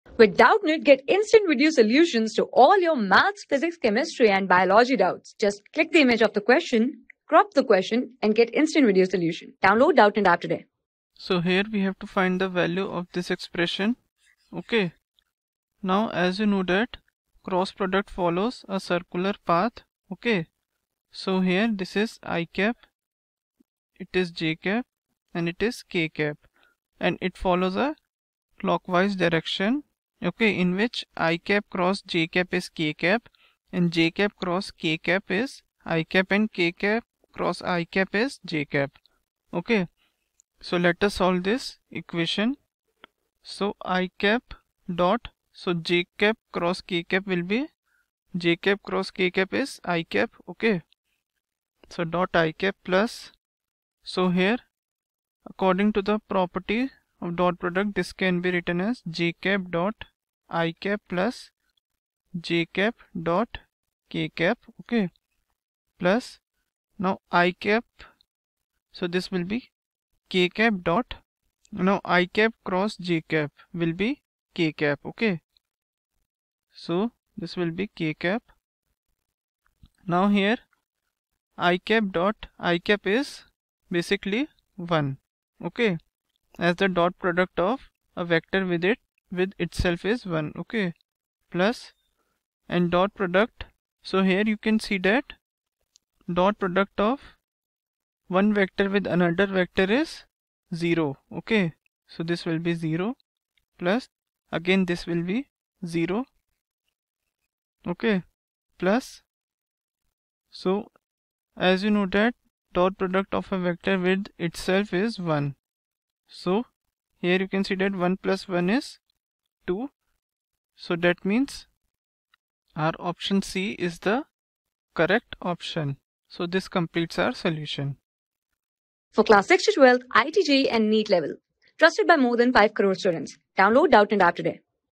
Thevalue of hat i .( hat jxx hat k)+ hat j.( hat i+ hat k)+ hat k.( hat ixx hat j)is(A)0(B) 1 (...
Thevalue of  hat i .( hat jxx hat k)+ hat j.( hat i+ hat k)+ hat k.( hat ixx hat j)is(A)0(B) 1 (C)1(D) 3
Class: 12
Subject: MATHS
Chapter: VECTOR OR CROSS PRODUCT
Board:CBSE

👉 Have Any Query? Ask Us.